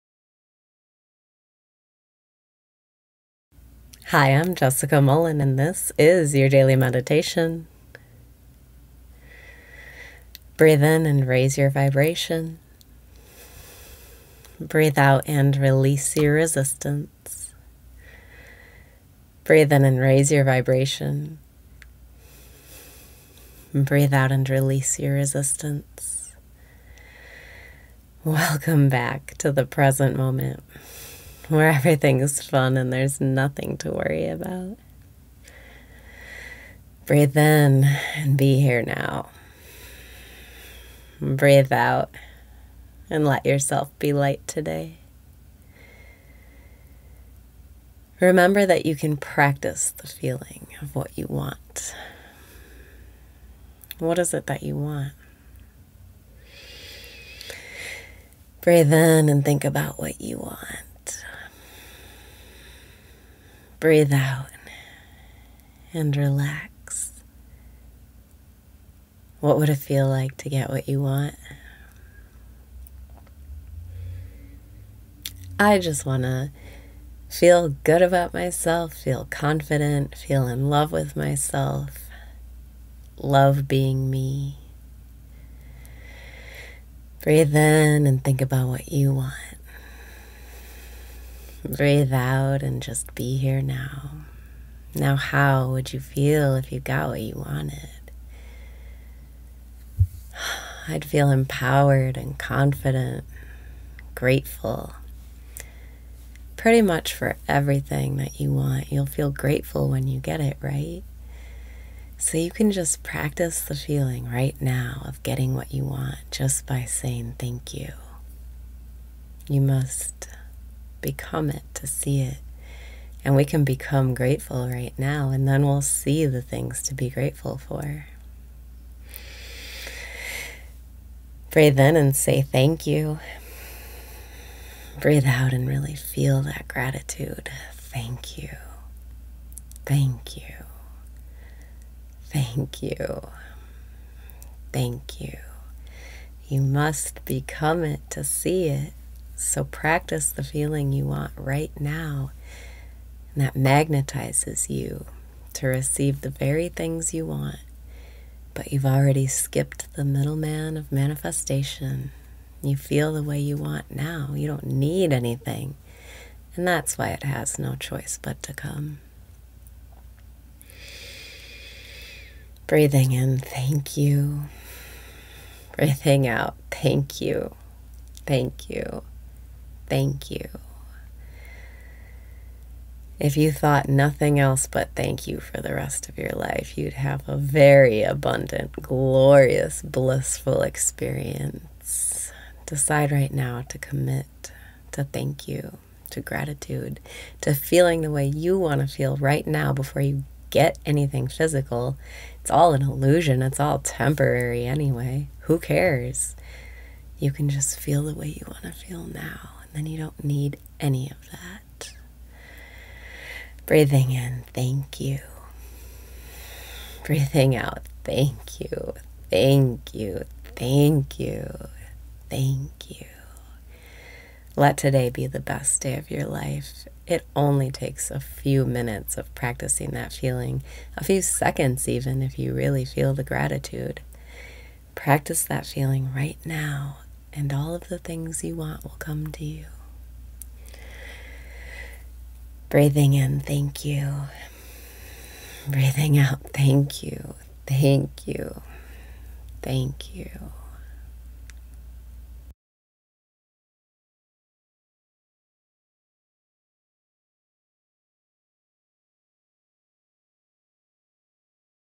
Daily Meditation - All You Want Will Come to You
Welcome back to the present moment, where everything’s fun and there’s nothing to worry about!

Breathe out and let yourself be light today.

Remember that you can practice the feeling of what you want. What is it that you want?

What would it feel like to get what you want? I just want to feel good about myself, feel confident, feel in love with myself—love being me.

Breathe out and just be here now.

Now how would you feel if you got what you wanted? I’d feel empowered and confident. Grateful. Pretty much for everything that you want, you’ll feel grateful when you get it, right? So you can just practice the feeling right now, of getting what you want, just by saying thank you. You must become it, to see it. And we can become grateful right now, and then we’ll see the things to be grateful for.

Breathe out and really feel that gratitude! Thank you. Thank you. Thank you. Thank you.

You must become it to see it! So practice the feeling you want, right now. That magnetizes you to receive the very things you want. But you’ve already skipped the middle man of manifestation. You feel the way you want now. You don’t need anything. And that’s why it has no choice but to come.

Thank you! Thank you! If you thought nothing else but “thank you” for the rest of your life, you’d have a very abundant, glorious, blissful experience. Decide right now to commit to thank you. To gratitude. To feeling the way you want to feel, right now, before you get anything physical. It’s all an illusion, it’s all temporary anyway. Who cares? You can just feel the way you want to feel now, then you don’t need any of that!

Thank you! Thank you! Thank you! Let today be the best day of your life. It only takes a few minutes of practicing that feeling—a few seconds really, if you really feel the gratitude. Practice that feeling right now and all of the things you want will come to you.